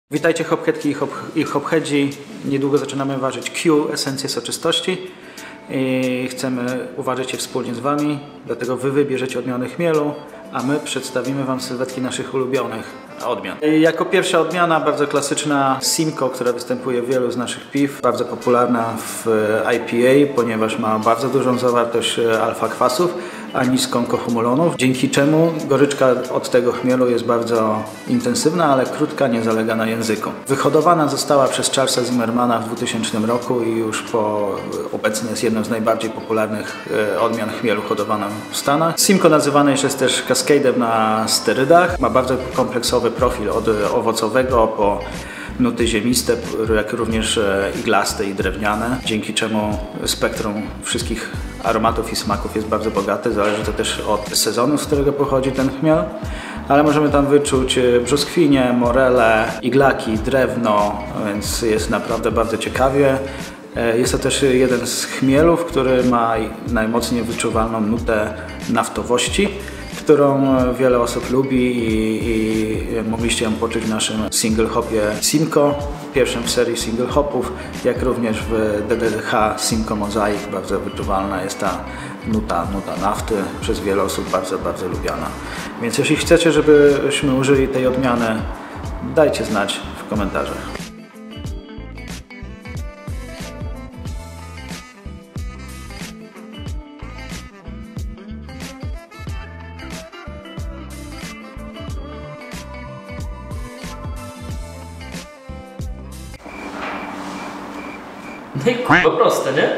Odmiany chmielu - Simcoe
Przygotowujemy się do uwarzenia poczwórnie soczystego piwa - QDH DIPA. Plan jest taki, aby to piwo było naszym wspólnym dziełem czyli AleBrowar + WY! W jaki sposób? Wy wybieracie chmiele, my warzymy i tak wspólnymi siłami stworzymy coś co ma poszerzyć naszą granicę pijalności! Na pierwszy ogień idzie chmiel SIMCOE!

Dawajcie znać w komentarzach jaki jest Wasz ulubiony chmiel lub też o jakim chcielibyście jeszcze się czegoś dowiedzieć!

►Jak nalewać i serwować piwo rzemieślnicze?